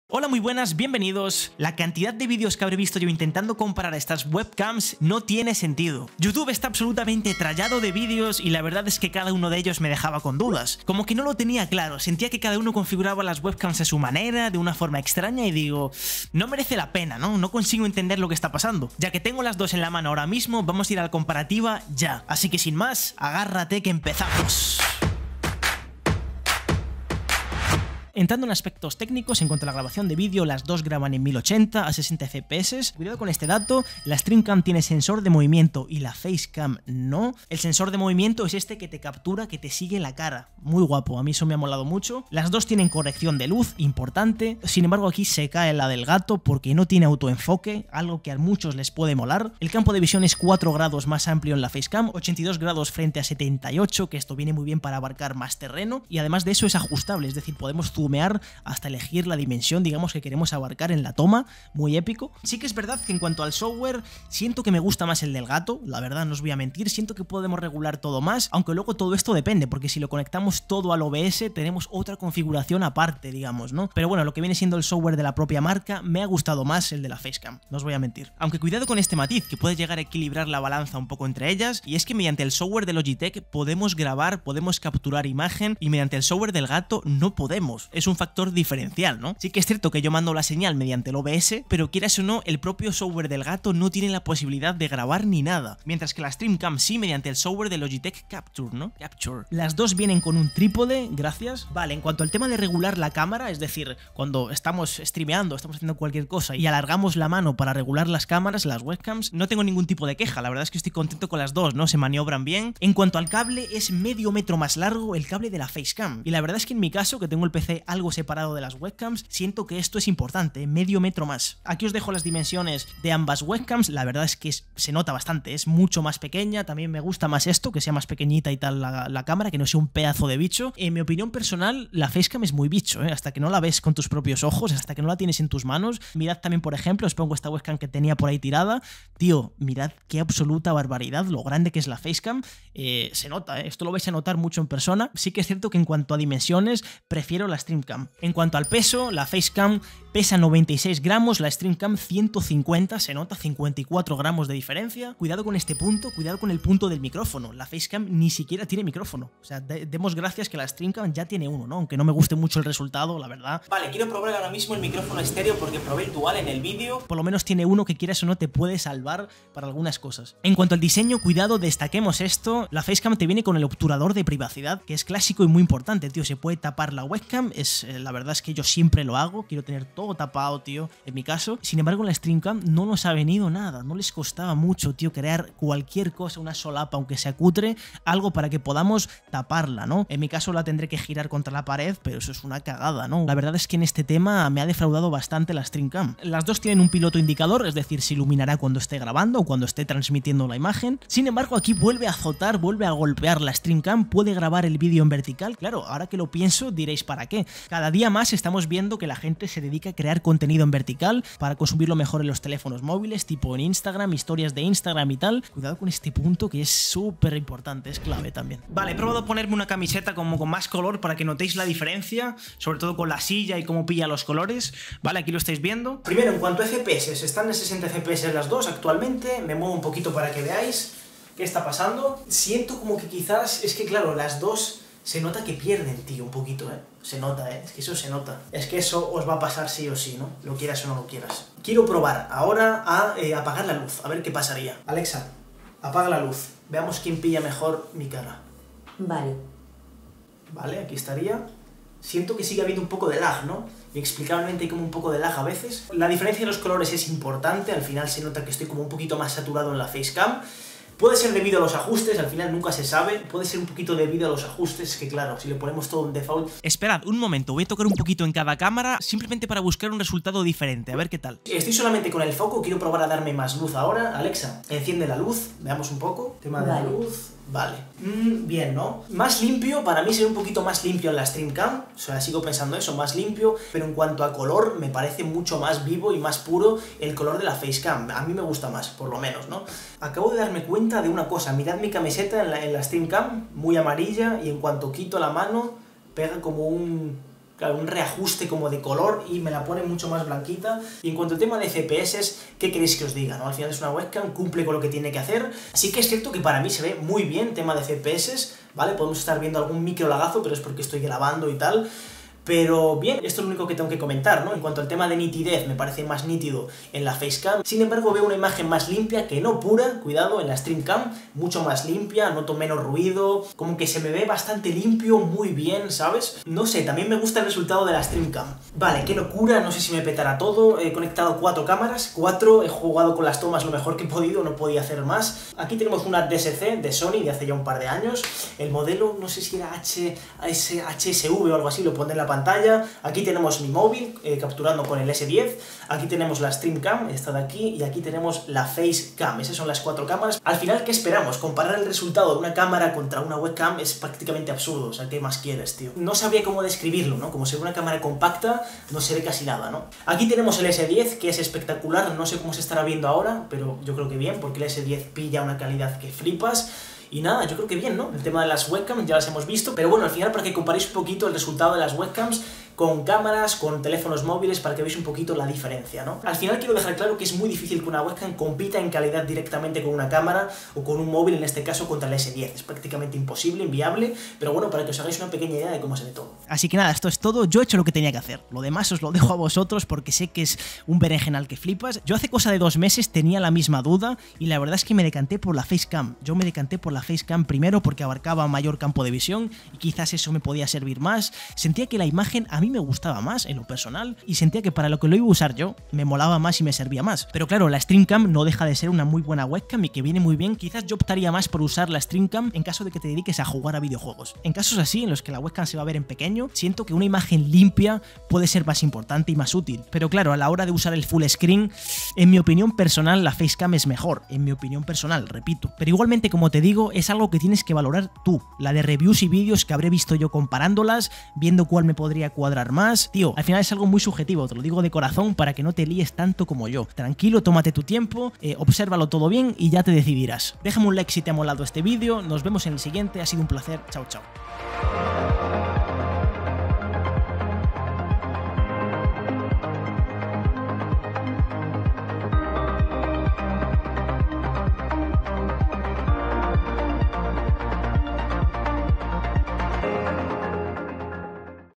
FACECAM vs STREAMCAM | 👉 16 Diferencias que DEBES SABER antes de comprar...
"Dos webcams que aportan cosas muy diferentes, pero ambas 1080p a 60 fps."

Logitech Streamcam
Elgato Facecam

0:00 Intro
0:22 16 Diferencias/Características
3:36 Facecam vs Streamcam
5:26 Comparativa con otros dispositivos
6:23 ¿Cuál compré primero?